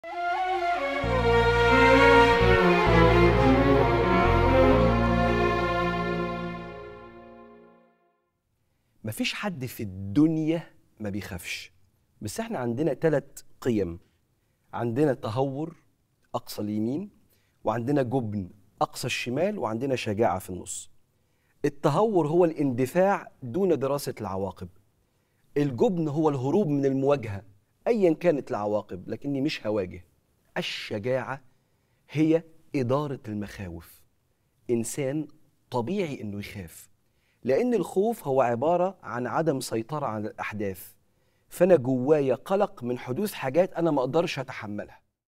في أقل من دقيقة.. اعرف الفرق بين التهور والجبن والشجاعة - مصطفى حسني
في أقل من دقيقة.. اعرف الفرق بين التهور والجبن والشجاعة..